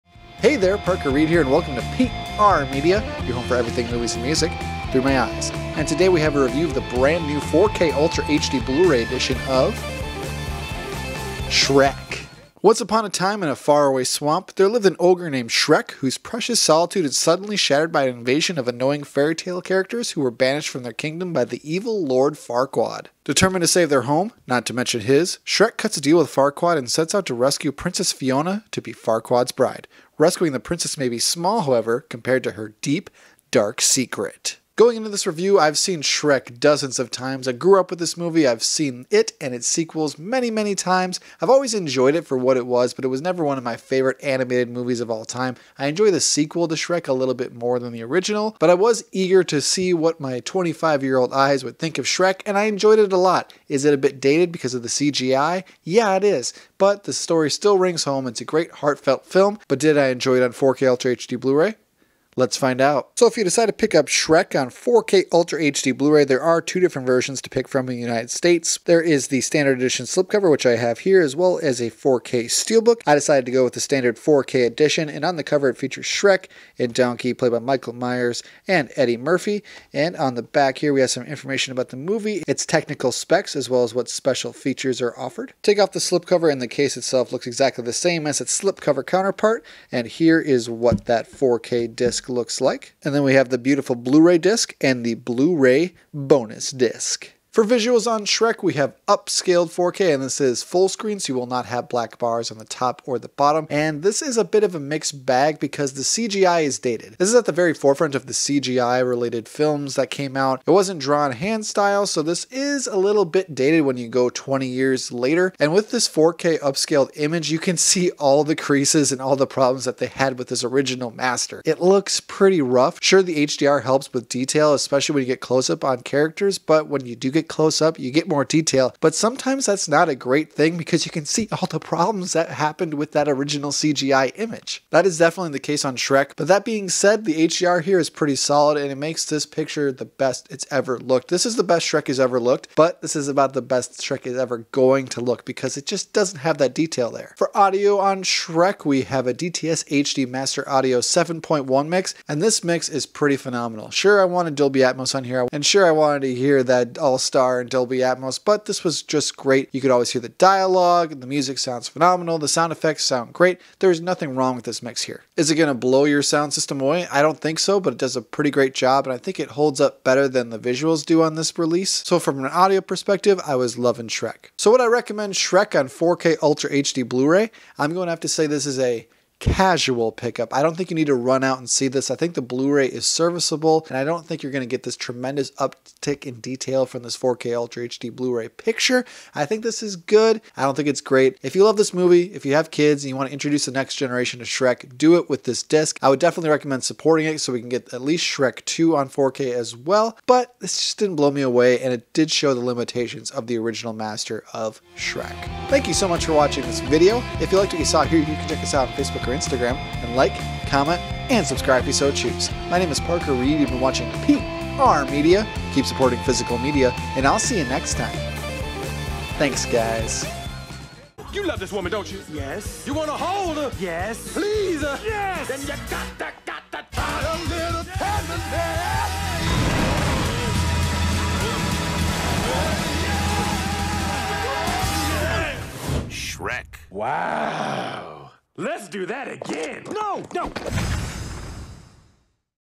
Shrek 4K Ultra HD Blu-ray REVIEW
Here I review the brand new 20th anniversary 4K Ultra HD Blu-ray edition of the classic animated fantasy movie, "Shrek." Does this piece of physical media deserve a place in your swamp? Let's find out...